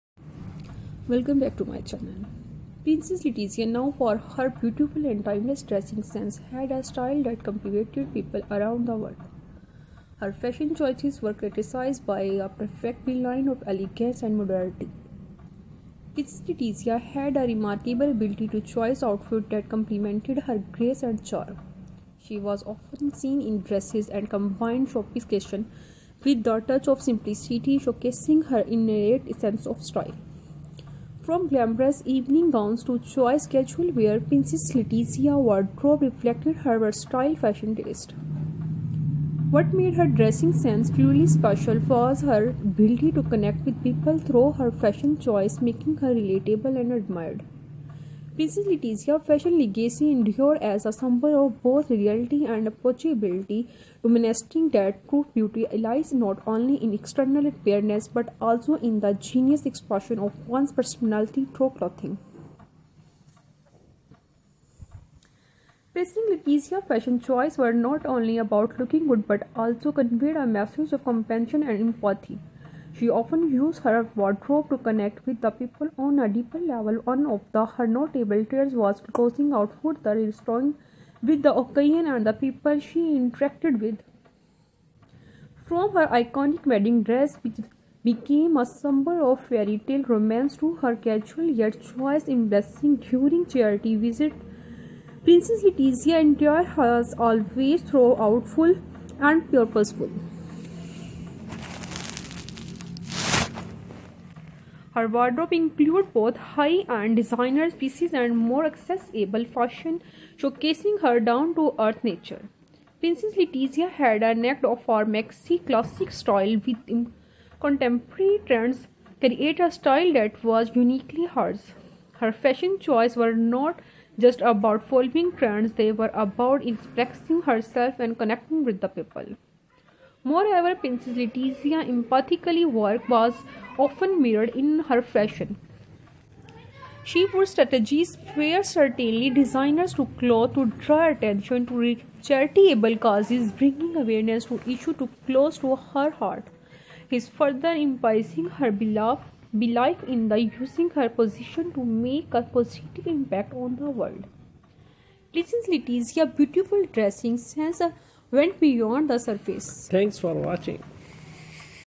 Princess Latizia Ortiz unveils her stor enchanting journey into the world of royal fashion
Princess Latizia Ortiz unveils her stor enchanting journey into the world of royal

Before meeting Prince Felipe VI, Ortiz worked as a television journalist. It’s rumored that after seeing her on television, the then-prince asked a mutual friend to set up a dinner for them to meet. Following their meeting in 2002, they began to date and announced their engagement in 2003. The couple have two daughters.

Prince Felipe VI of Spain, born Felipe Juan Pablo Alfonso de Todos los Santos de Borbón y Grecia on January 30, 1968, is the current king of Spain. He ascended to the throne on June 19, 2014, following the abdication of his father, King Juan Carlos I.

Letizia Ortiz Rocasolano, born on September 15, 1972, in Oviedo, Spain, became Queen Letizia upon her marriage to Felipe in 2004. Before her marriage, she worked as a journalist and news anchor. Queen Letizia is known for her elegance and modern approach to her role.

King Felipe VI and Queen Letizia have two daughters, Princess Leonor, born on October 31, 2005, and Infanta Sofía, born on April 29, 2007. The family is often seen carrying out official duties, representing Spain at various events, and participating in charitable activities.

As the Spanish royal couple, Felipe and Letizia are respected for their commitment to public service, and they strive to connect with the Spanish people in a relatable and approachable manner. Their reign has marked a period